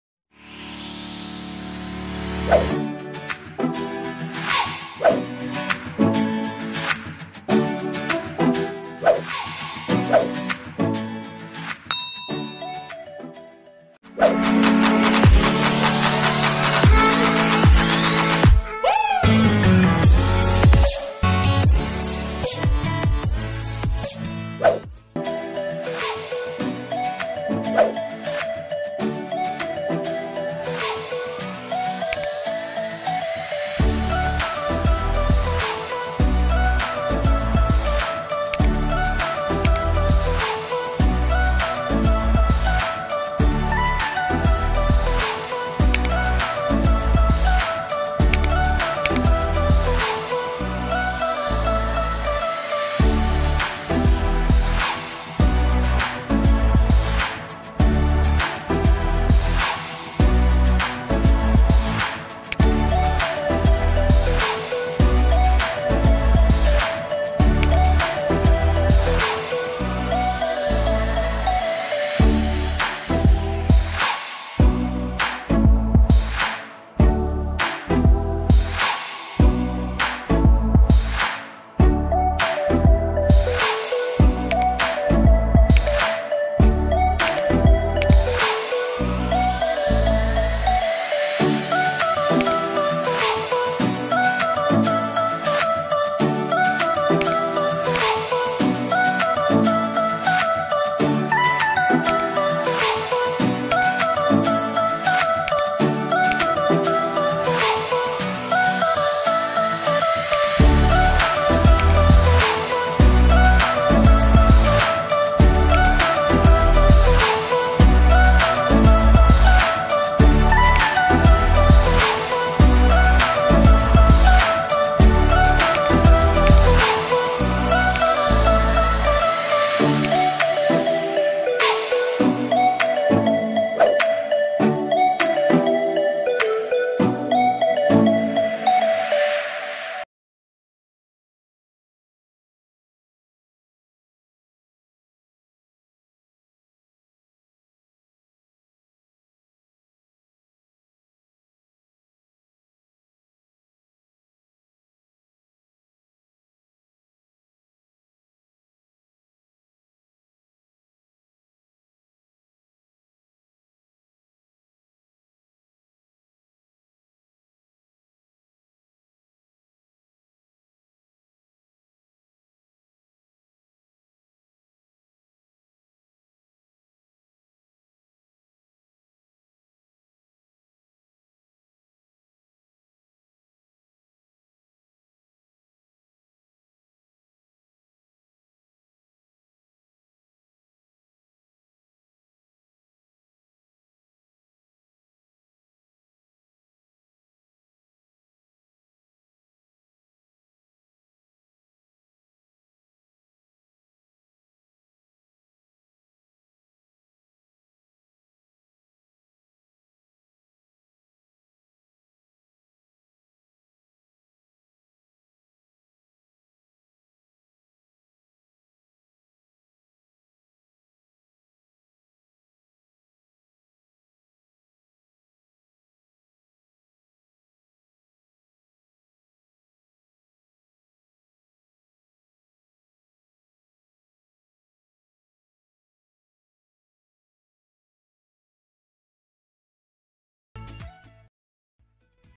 How to create text audio spectrum using aver player in Android. | Avee player / Pixellab
In this video we know how to creat text audio spectrum using avee player in android.